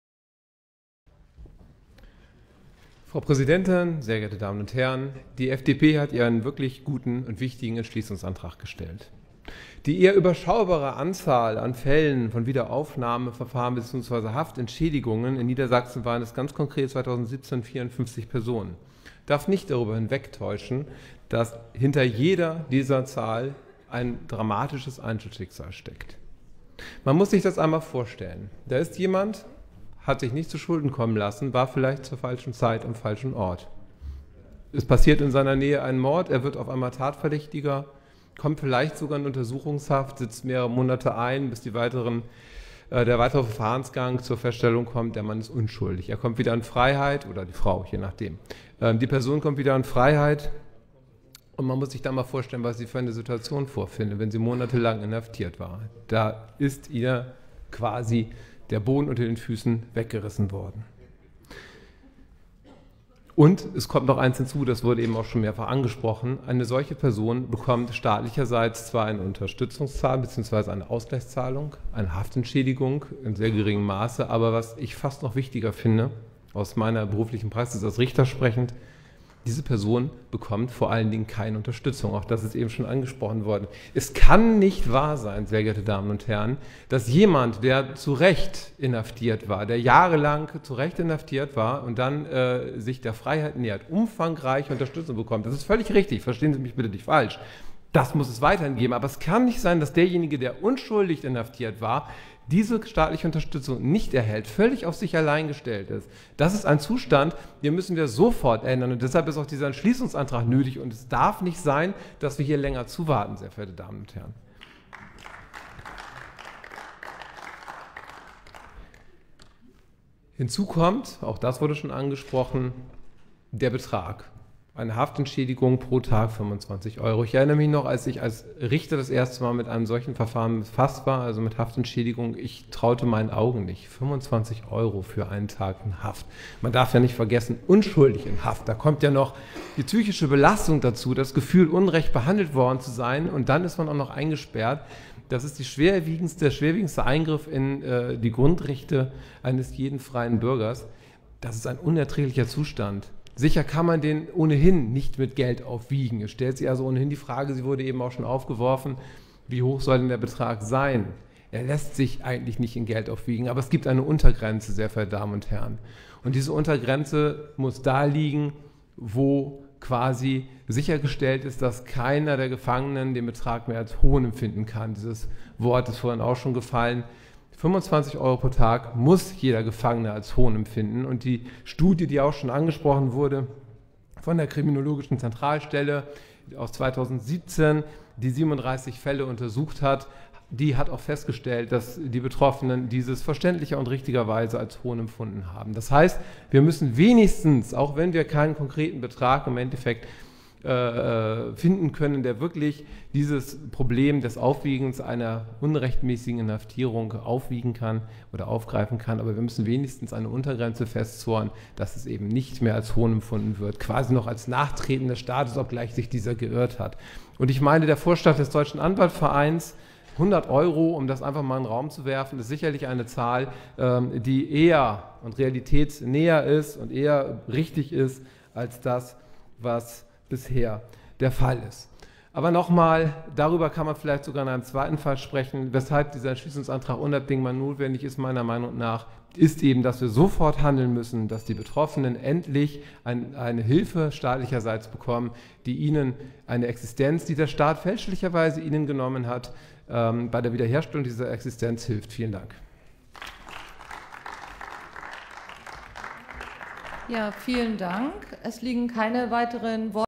Christopher Emden, MdL (AfD) zum Thema Haftentschädigungen (25.01.2018)
Erste Beratung

Die Haftentschädigung muss erhöht werden! - Spezielle Betreuungsangebote einführen!

Antrag der Fraktion der FDP - Drs. 18/151